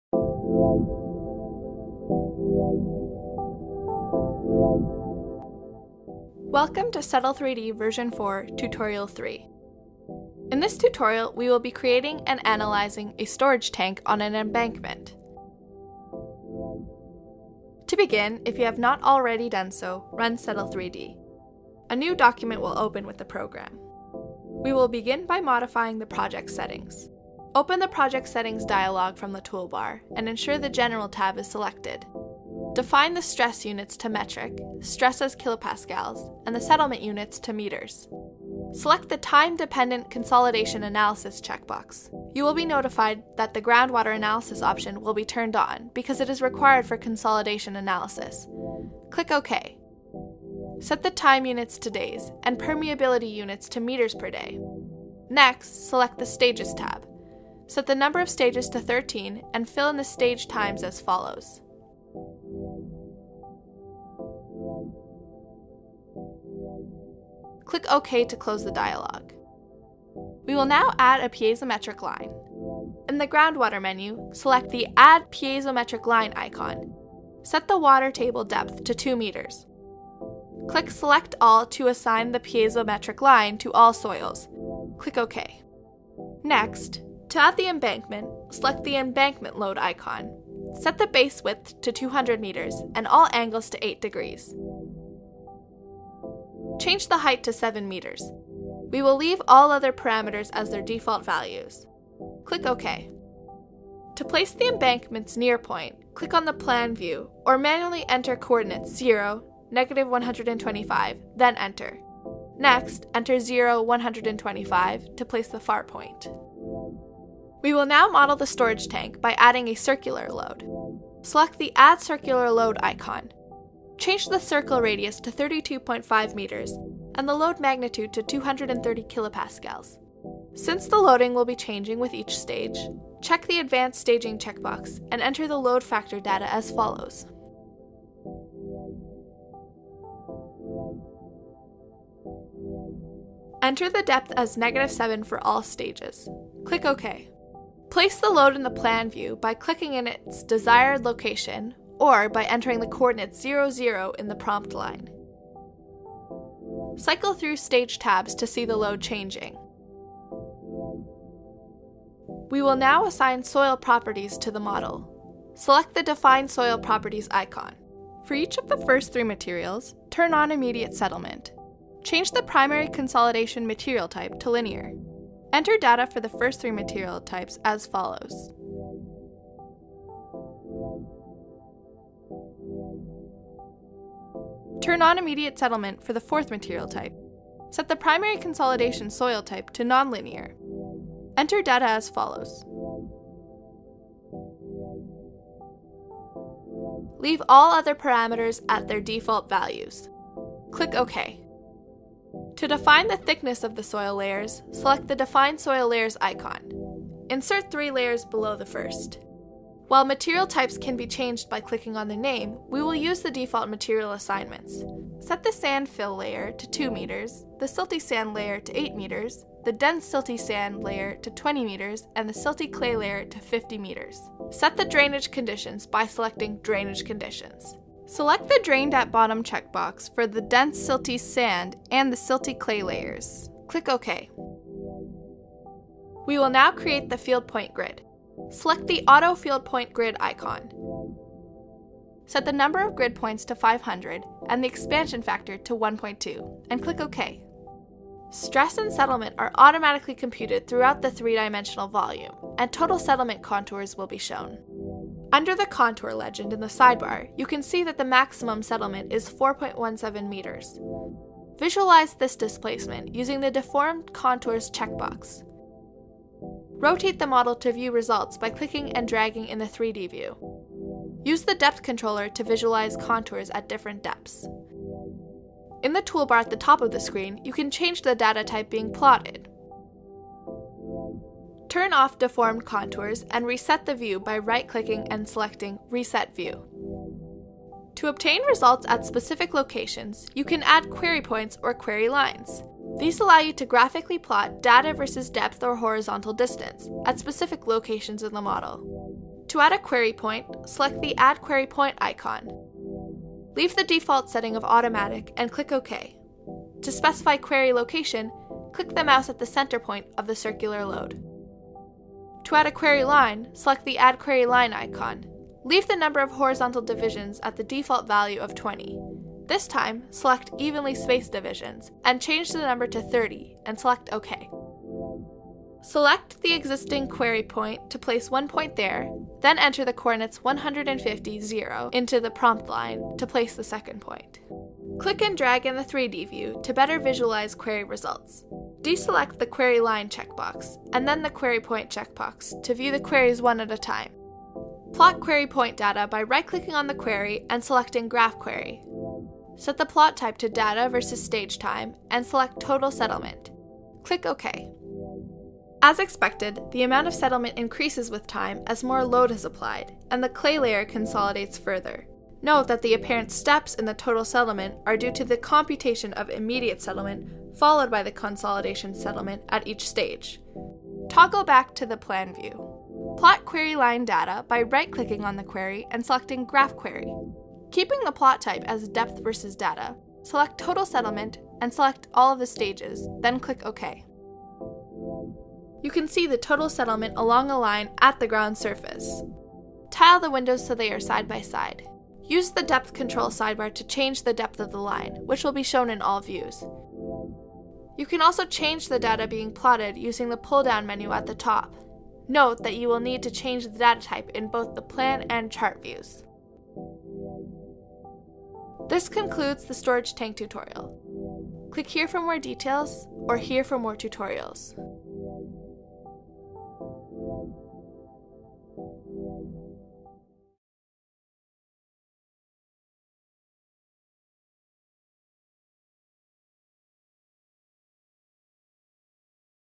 Settle3D Tutorial 3 - Storage Tank on Embankment
In the Settle3D Storage Tank tutorial, we introduce load staging, embankments, multiple materials, and time-dependent consolidation analysis. For more information on Settle3D, see the following links: